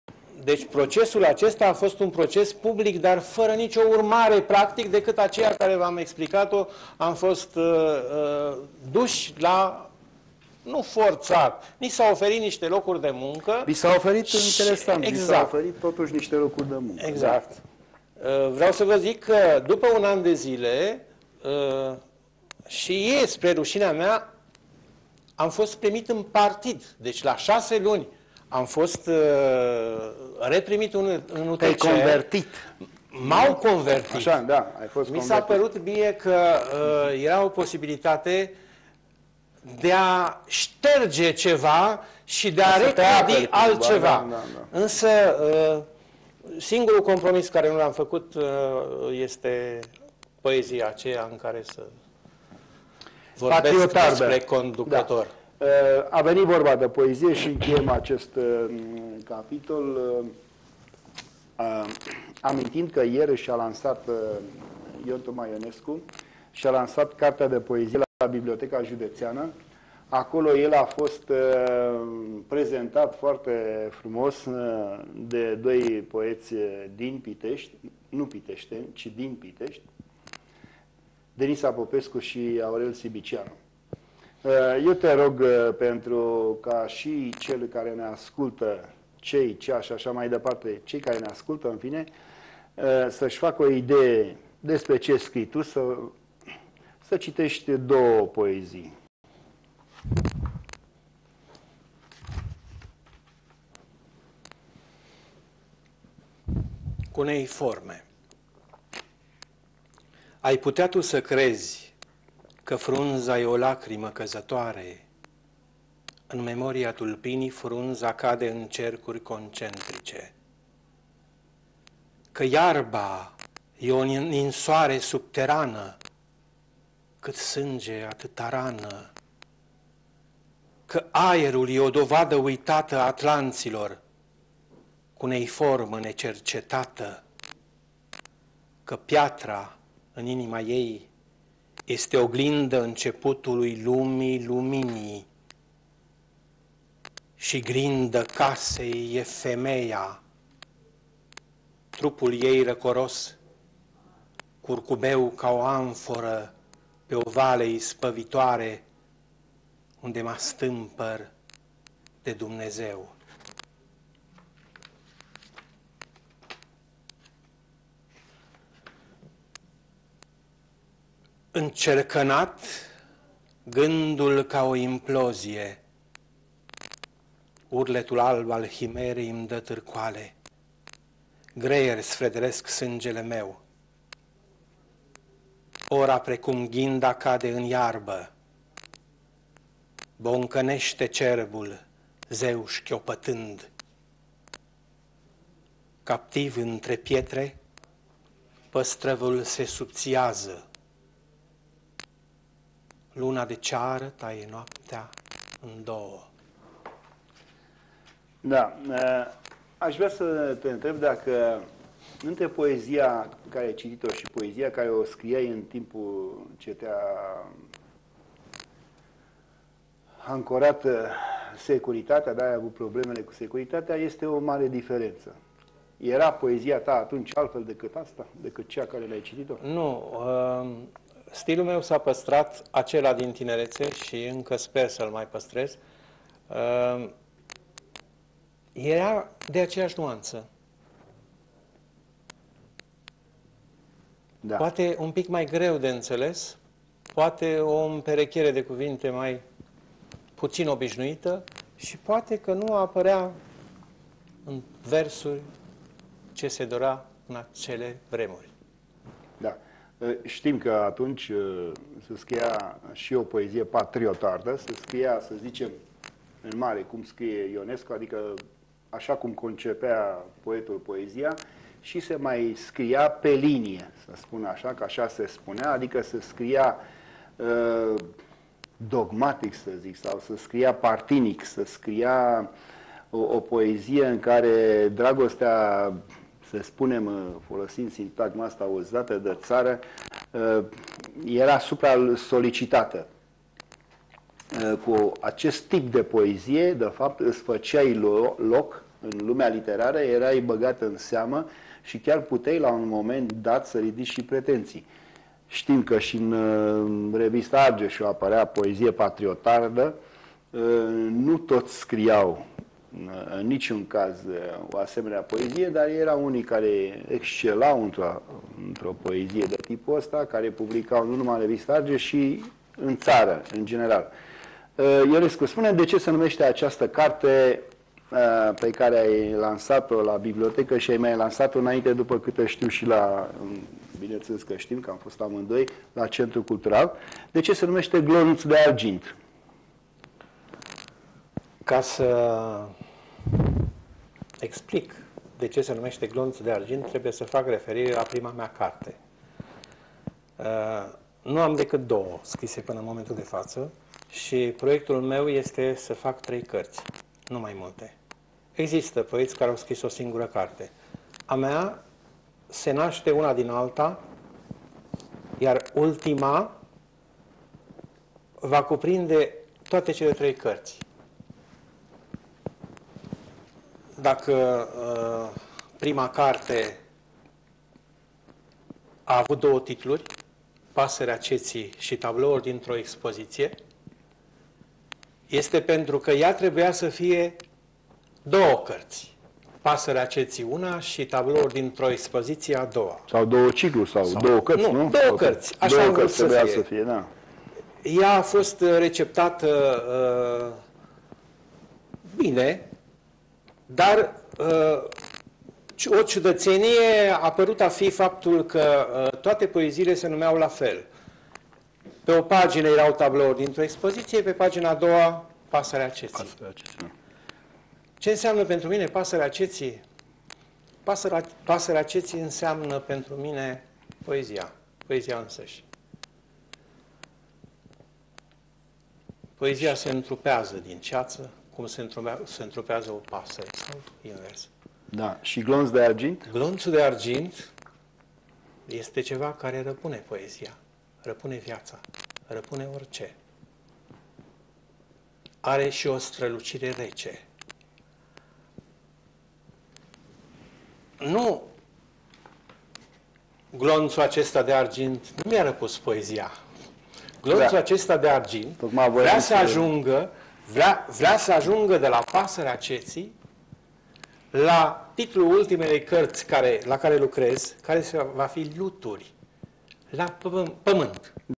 Ora Scriitorului_06
Virgil Diaconu de vorba cu Ion Toma Ionescu